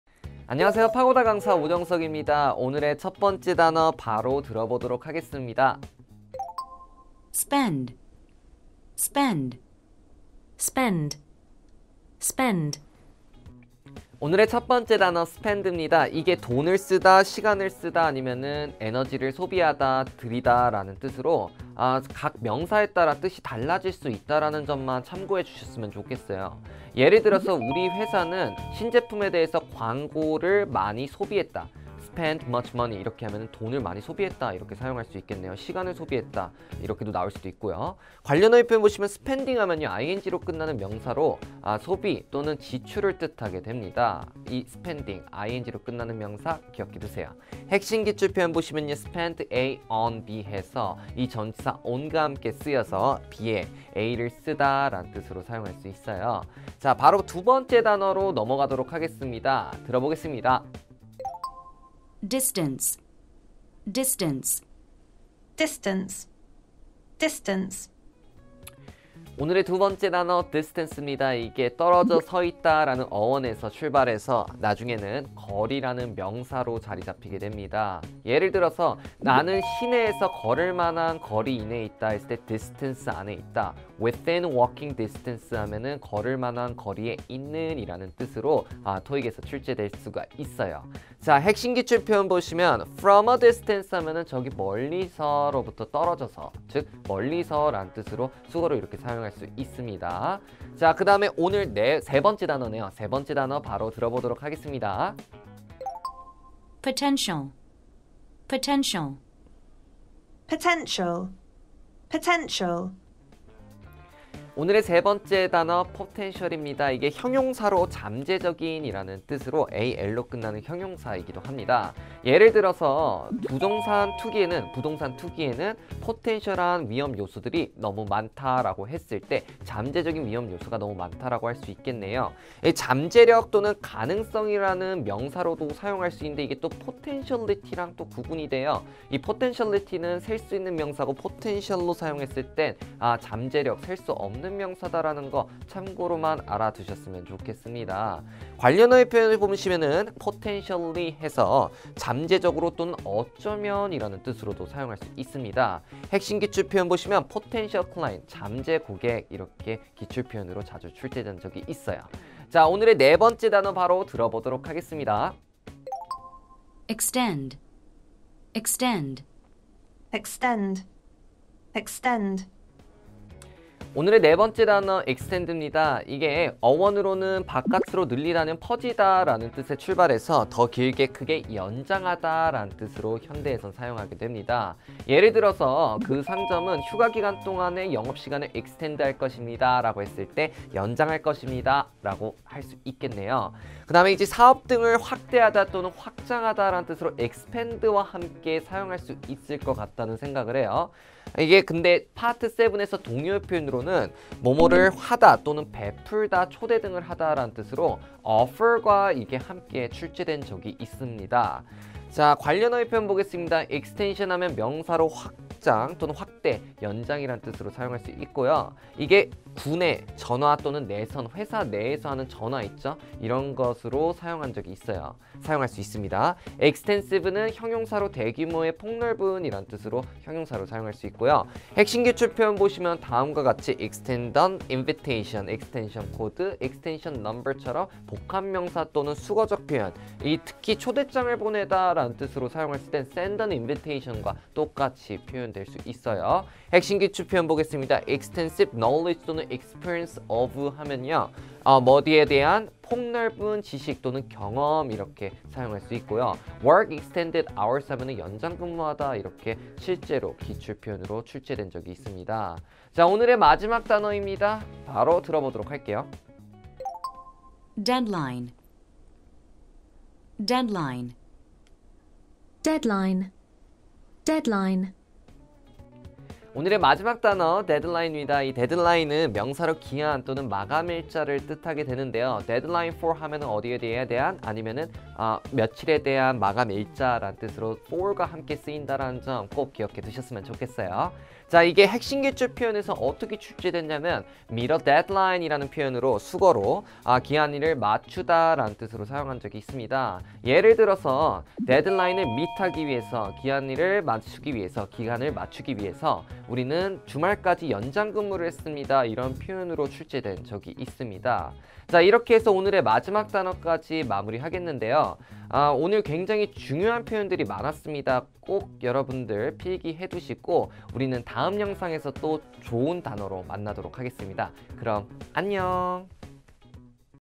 [DAY 04 03강] ★파고다 토익 VOCA★ 우선순위 영단어: '마감일을 맞추다'를 영어로 하면?
토익 단어, 지루하게 외우지 마세요~ 파고다 훈남♥ 강사 오정석 쌤이 깔끔★상쾌★유쾌하게 요점만 콕 집어주는 [파고다 토익 VOCA] 우선순위 영단어 1500!

파고다 토익 VOCA의 철저한 기출 분석으로 뽑아낸 우선순위 영단어 1500개 완전 정복으로 토익 어휘 문제를 마스터해 보세요!!

★ 오늘의 어휘 ★
spend (돈을) 쓰다; (시간을) 보내다; (에너지, 노력 등을) 들이다
distance 거리
potential 잠재적인; 잠재력, 가능성
extend 연장하다, 확대하다, (환영 등을) 베풀다
deadline 기한, 마감 일자